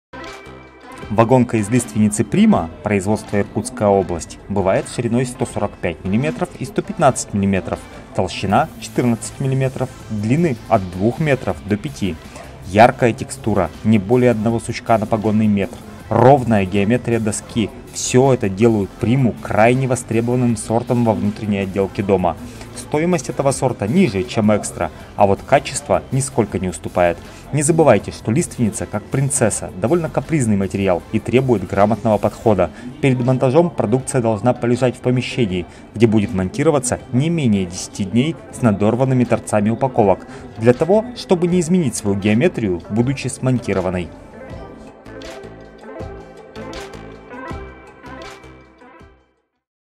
Вагонка из лиственницы, 115, сорт прима (А)
Друзья!
В этом видео мы расскажем вам о вагонке из лиственницы в сорте Прима

Подробности по телефону:
Заходите на наш сайт: лесобаза.рф